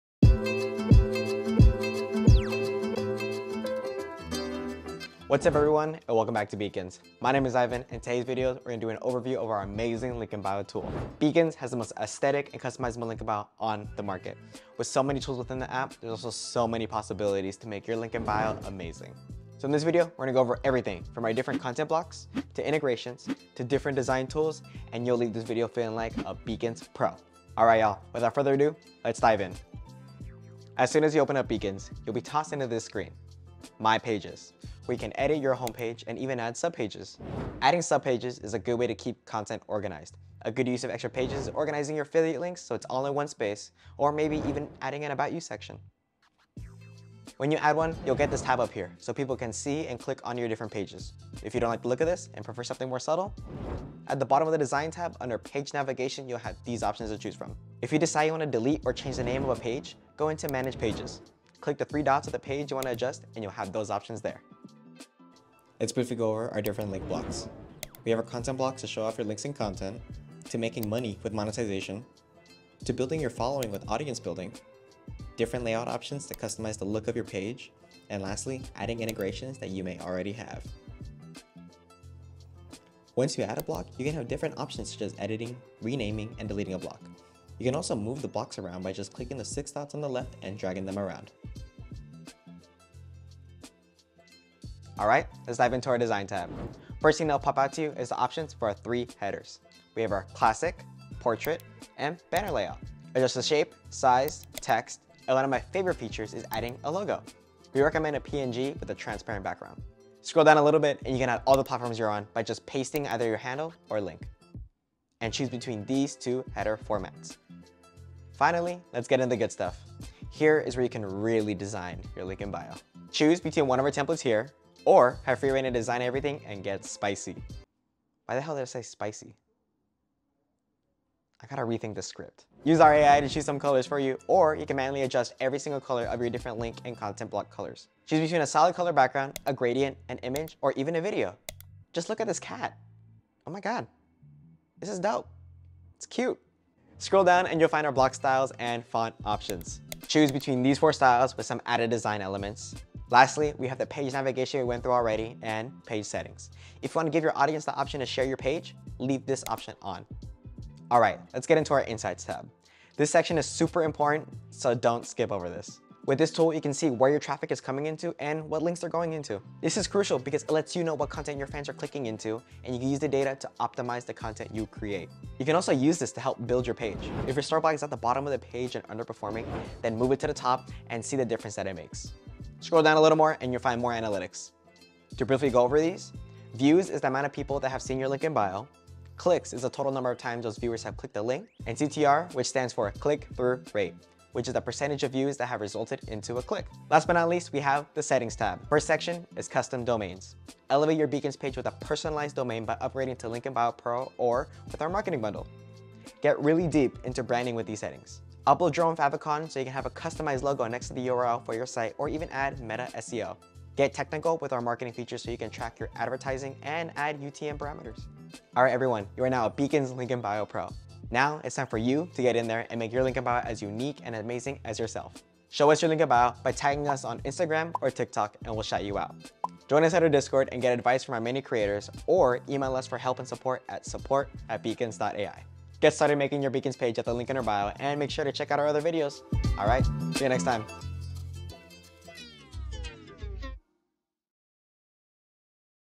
Beacons Link in Bio (FULL OVERVIEW)
With so many tools ⚒️ and options we have created this video to help you make your beacons page as amazing and aesthetic as yourself 🫵!

In this video we are going to demonstrate how to create and design your Beacons Link in Bio as well as managing pages, all our different content, monetization, audience building, and integration blocks! As well as some in depth explanations of our insights and settings tab!

🕐  Timestamps  🕐

0:00 - Introduction 🏁
0:19 - Overview
0:30 - Managing Pages 📄
1:06 - Link Blocks 🔗
01:35 - Designing your LIB 🎨
02:40 - Link in Bio Insights 📈
03:16 - Settings Tab ⚙️
03:38 - Ending 🎬